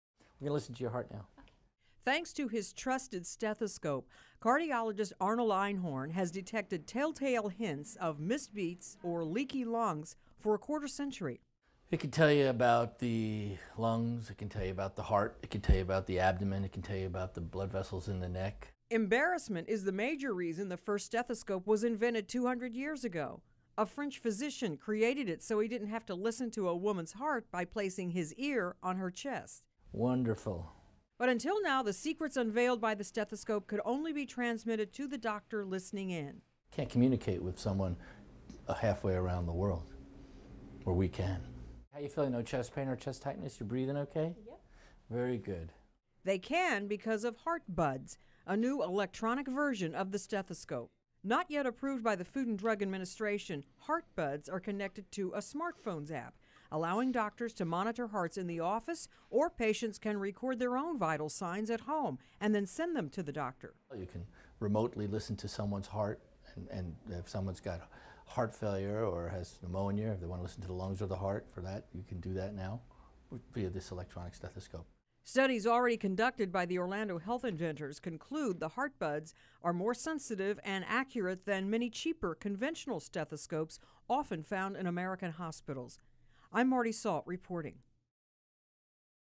Heartbuds: A Remote Stethoscope
An updated version of the traditional stethoscope allows doctors to monitor the hearts of patients from the office or from half-way around the world